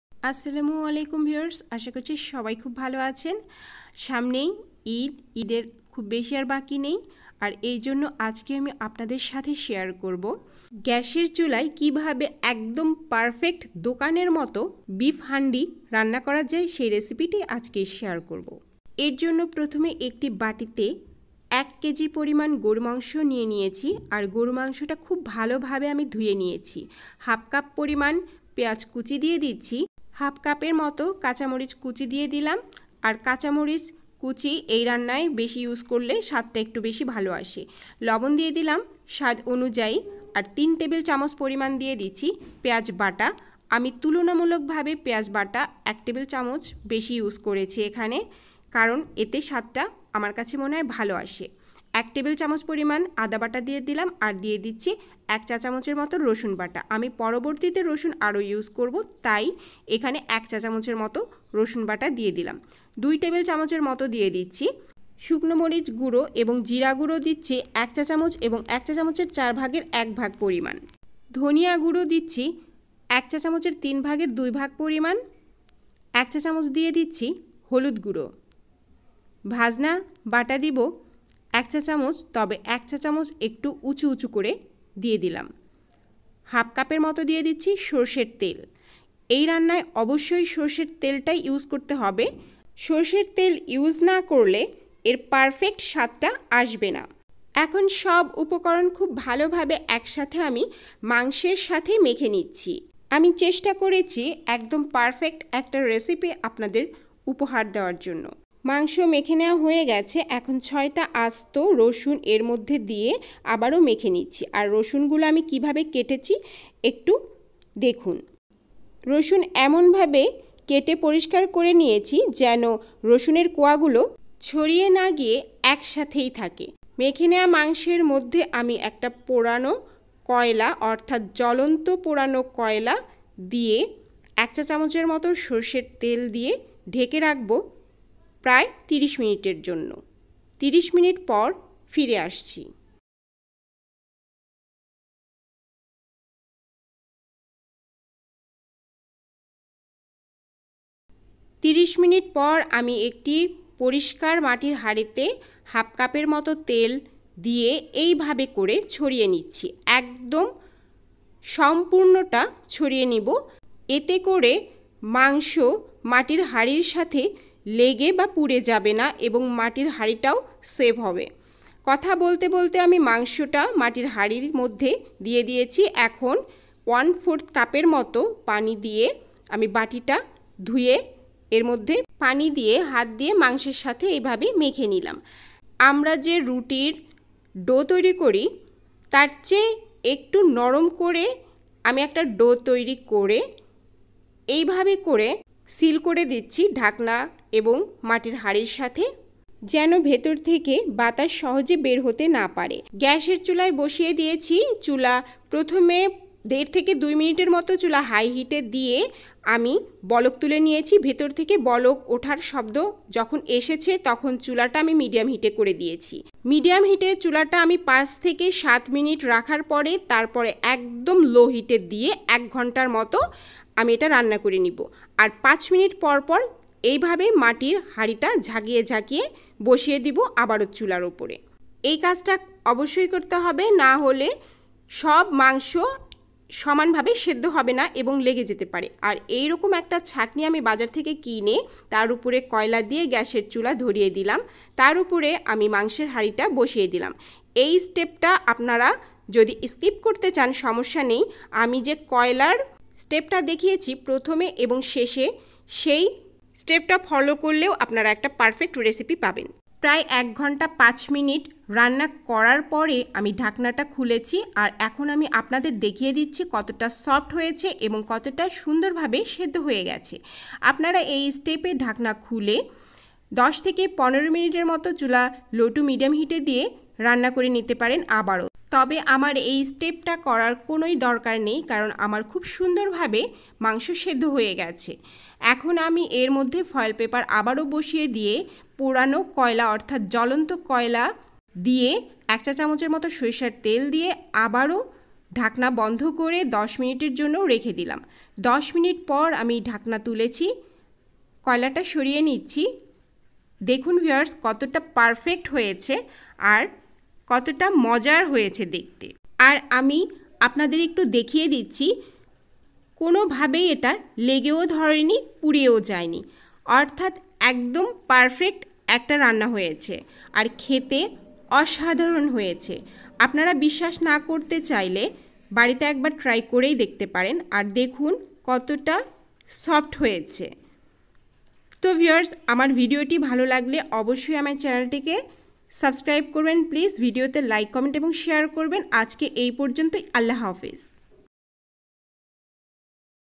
গ্যাসের চুলায় একদম হোটেলের পারফেক্ট বিফ হান্ডি রেসিপি/মাটির হাড়িতে গরু মাংস।Tasty Recipes by Rashma.
গ্যাসের চুলায় একদম হোটেলের পারফেক্ট বিফ হান্ডি রেসিপি/মাটির হাড়িতে গরু মাংস।এই রেসিপিতে গরু মাংস রান্না করলে অসম্ভব মজার হয়।একবার কেউ খেলে বারবার রিকুয়েস্ট করবে আবারো রান্না করার জন্য।